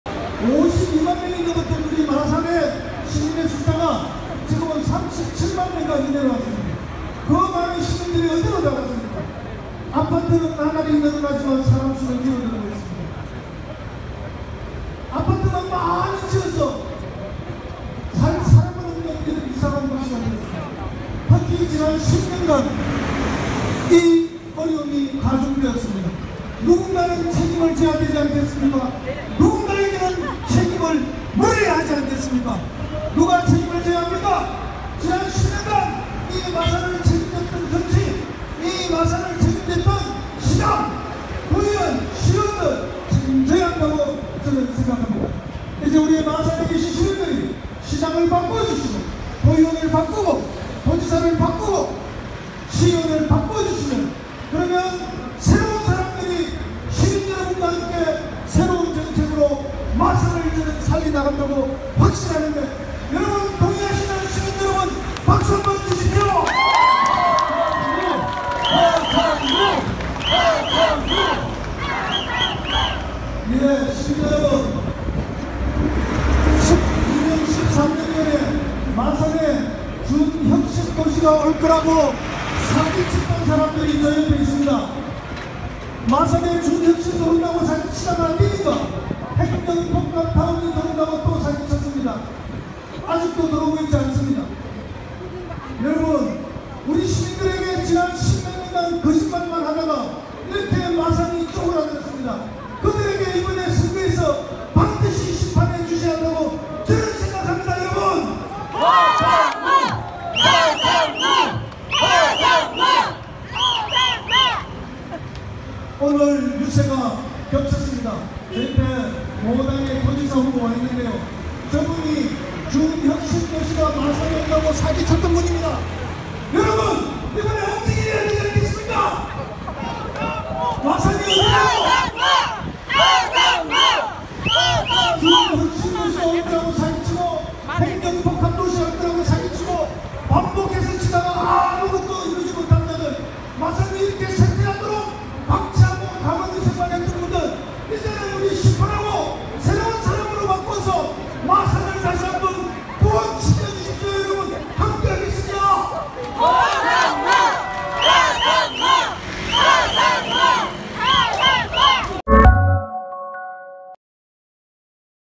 김태호 후보가 나타나자 허성무 창원시장 후보가 보인 격한 반응
공식선거운동 첫날 더불어민주당 허성무 후보가 마산어시장에서 유세를 열었다. 그런데 마침 인근에서 자유한국당 김태호 경남도지사 후보도 유세를 하기 위해 나타났다.
김태호 후보를 본 허성무 후보는 격한 목소리로 그를 성토했다.